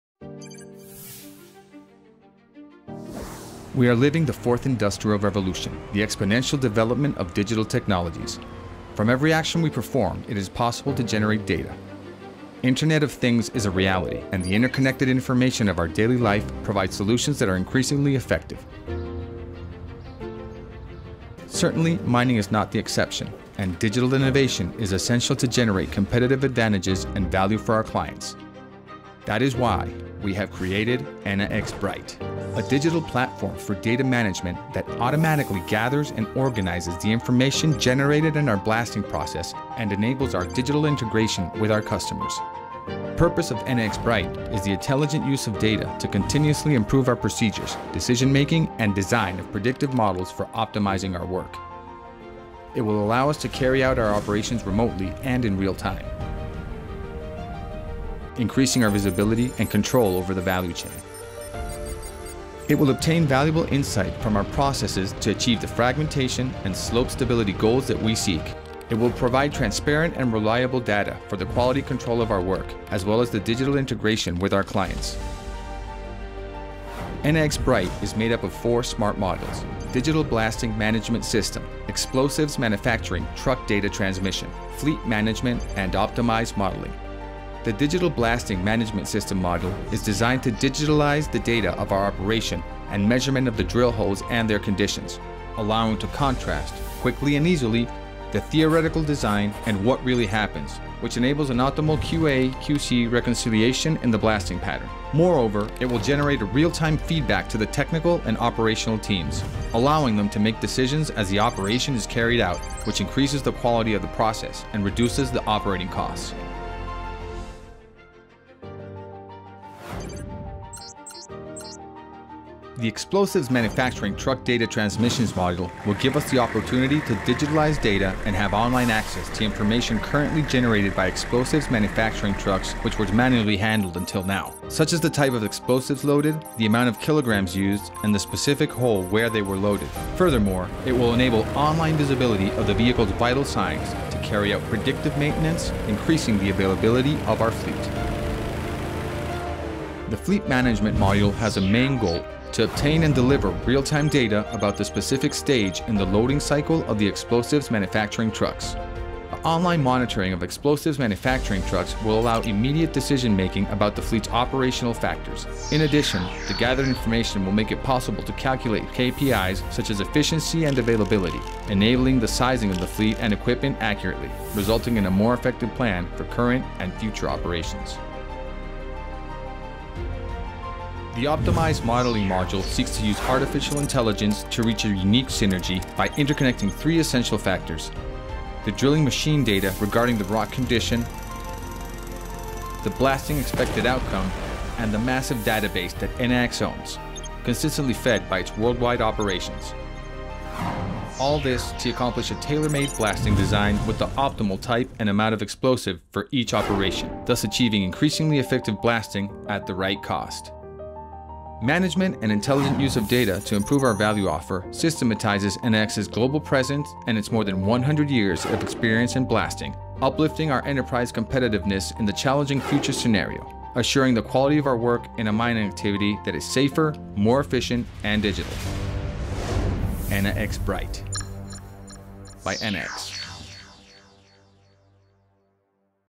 Enaex Bright - Digital data management platform
We have created Enaex bright, a digital platform for data management that automatically gathers and organizes the information generated in our blasting process in order to continuously improve our procedures, decision making and design of predictive models. ​